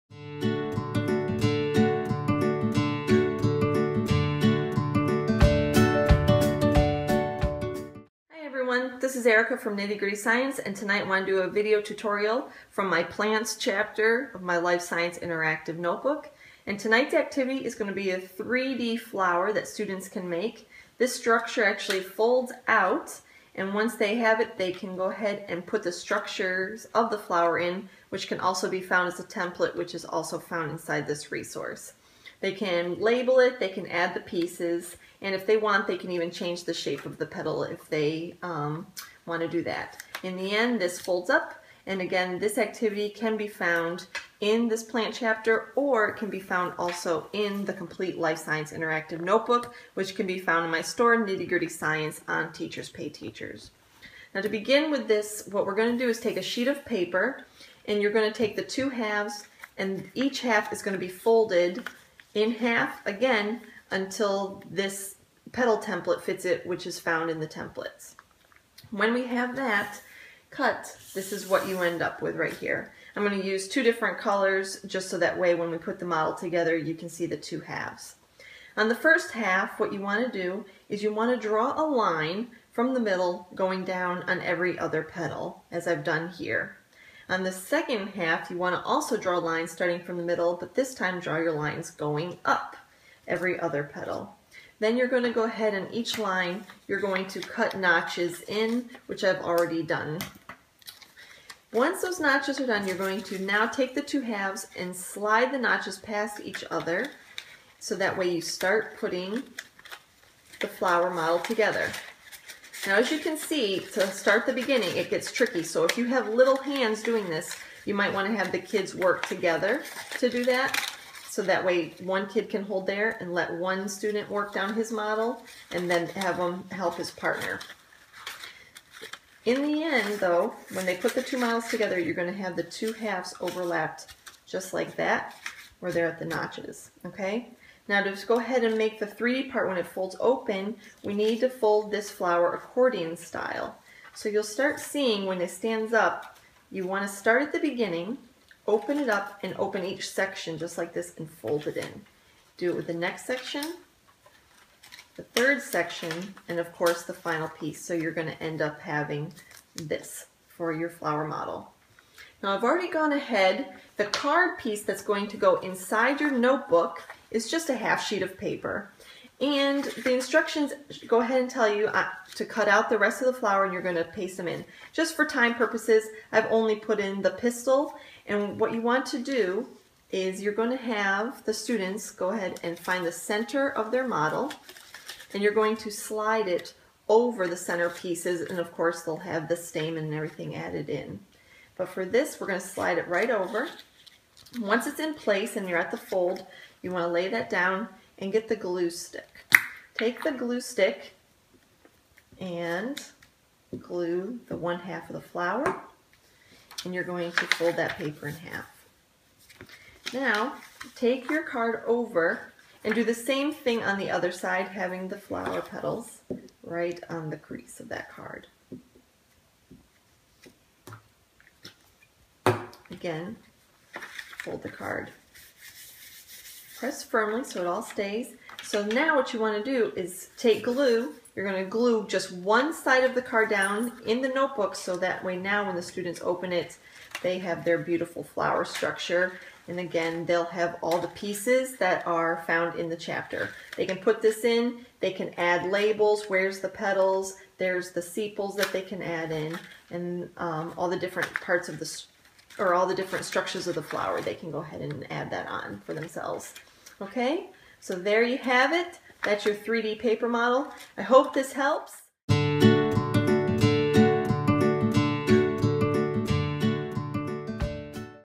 Science Interactive Notebook - 3D Flower
This video is a tutorial on how to make my 3D flower model for science interactive notebooks.